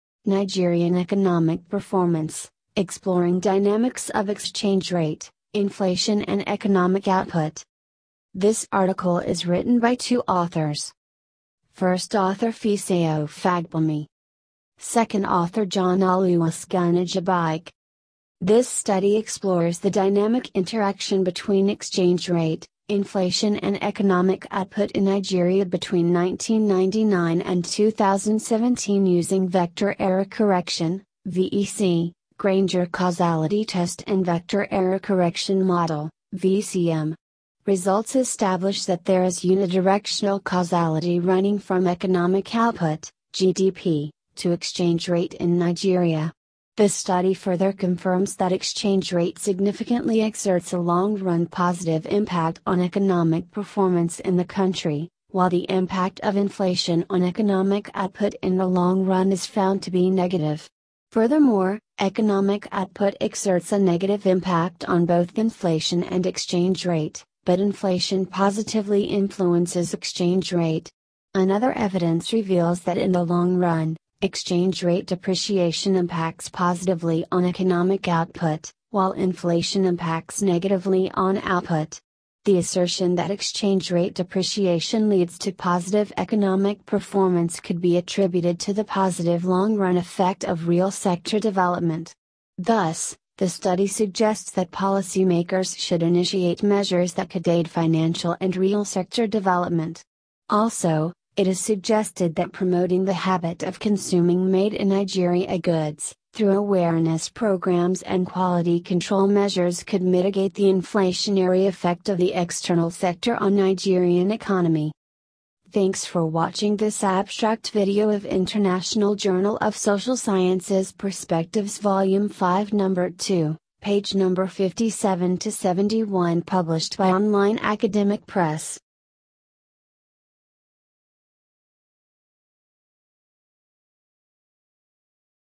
Nigerian Economic Performance Exploring Dynamics of Exchange Rate, Inflation and Economic Output IJS
Nigerian Economic Performance: Exploring Dynamics of Exchange Rate, Inflation and Economic Output